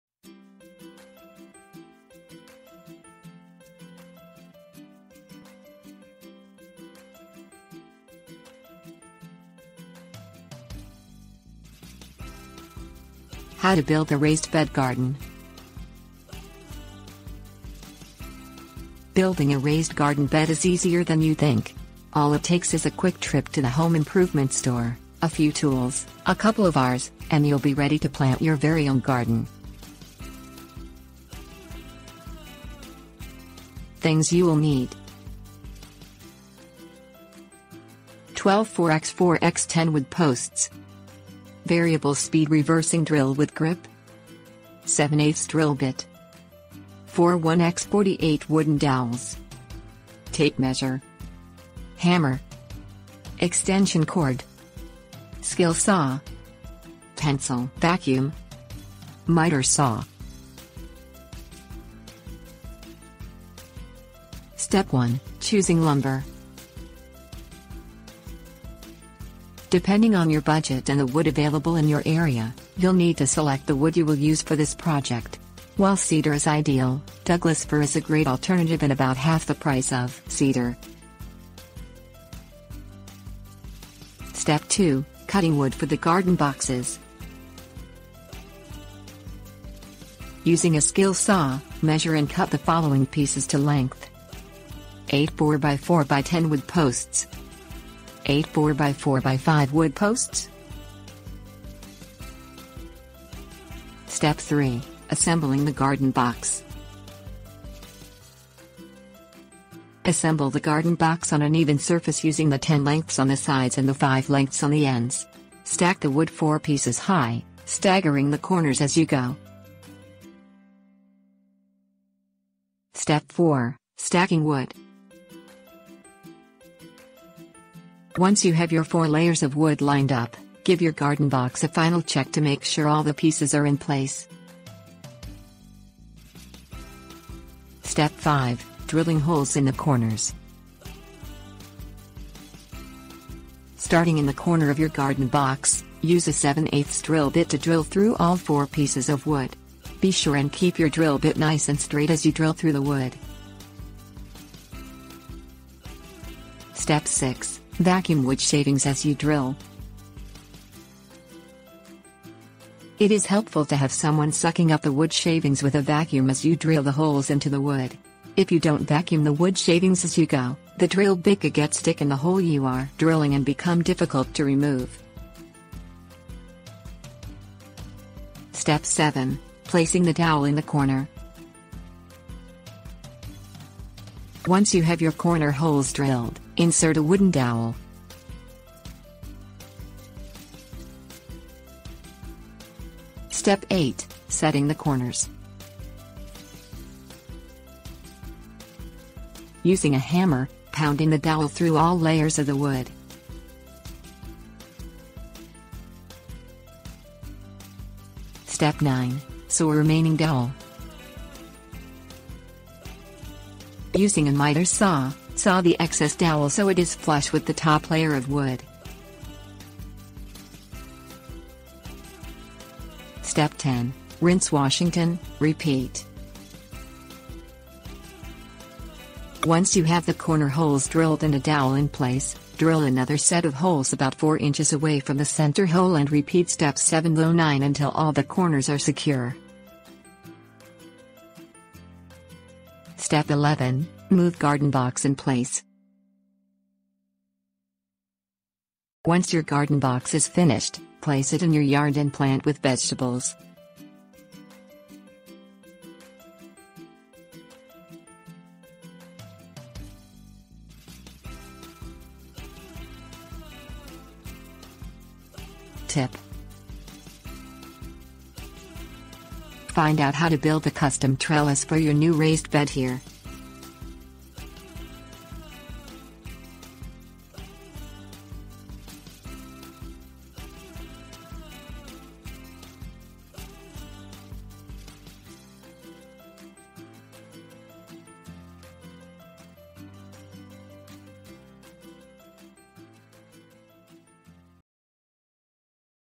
How to Build a Raised Bed Garden
Build a raised garden bed with just a few common household tools in a couple of hours. Growing food has never been so much fun.

Table of contents How to Build a Raised Bed Garden
Things You'll Need 00:32
Step 1: Choosing Lumber 01:03
Step 2: Cutting Wood For The Garden Boxes 01:23
Step 3: Assembling The Garden Box 01:43
Step 4: Stacking Wood 02:02
Step 5: Drilling Holes in the Corners 02:17
Step 6: Vacuum Wood Shavings as You Drill 02:36
Step 7: Placing the Dowel in the Corner 02:58
Step 8: Setting the Corners 03:13
Step 9: Saw Remaining Dowel 03:28
Step 10: Rinse, Wash, Repeat 03:43
Step 11: Move Garden Box in Place 04:03